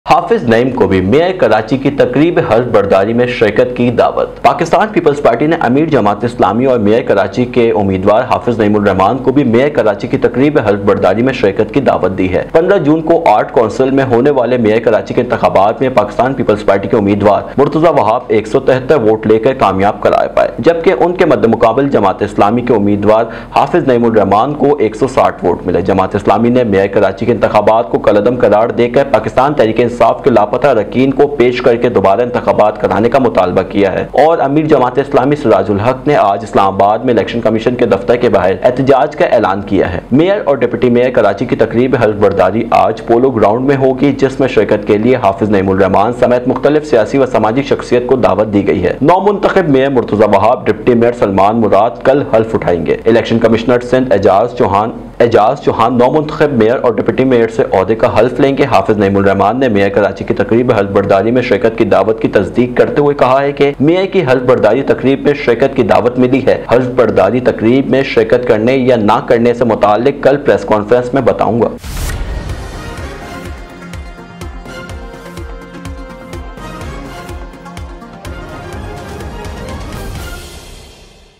Hafiz Naeem Was Also Invited to Participate in Oath-Taking Ceremony of Mayor Karachi|Speed ​​92 News
حافظ نعیم کو بھی میئر کراچی کی تقریب حلف برداری میں شرکت کی دعوت | اسپیڈ 92 نیوز